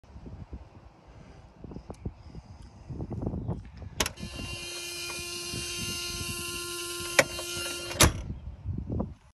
Original Key-in-car Buzzer
Car still retains the original key-in-car buzzer, very rare!